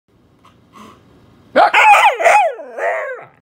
Hand Puppet Vs Husky
Banner the Siberian Husky takes on the vicious and wild hand puppet that barks